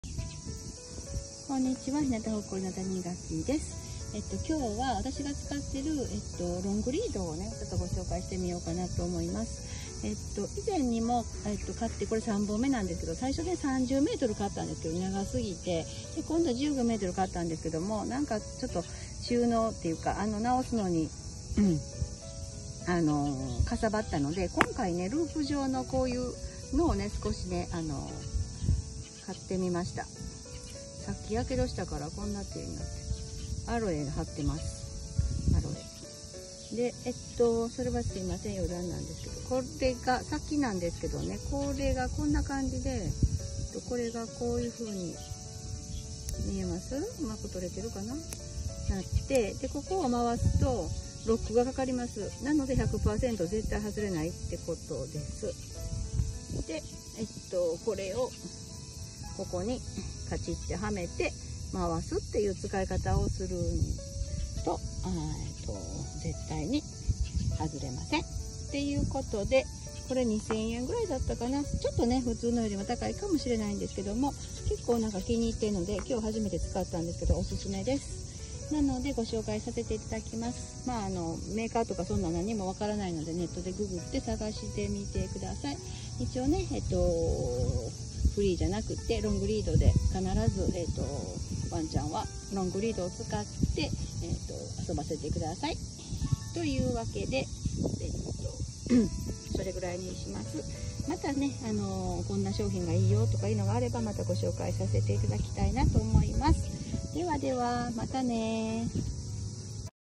【ロングリード】お気に入り、オススメの紹介
お気に入りのロングリードの紹介
ロングリード
3本目なんです
1本目30メートルは
長すぎた
15メートルの
平たいヒモの分
まとまらない
結果
ループのもの
10 メートルが
お気に入り
参考に
なれば嬉しいな